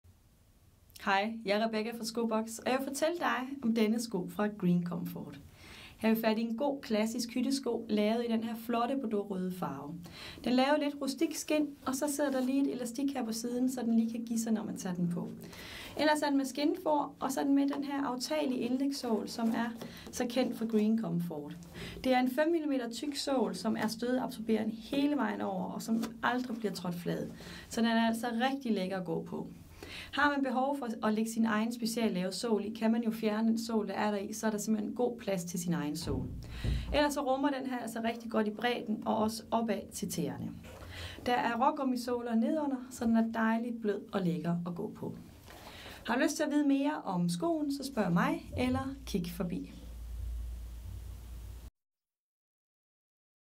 Green Comfort - Klasssisk skind Sko, Bordeaux Læder Sko
Flot klassisk sko fra Green Comfort i bordeaux farvet skind. Skoen er lige til at smutte i med elastik ved vristen og den rum...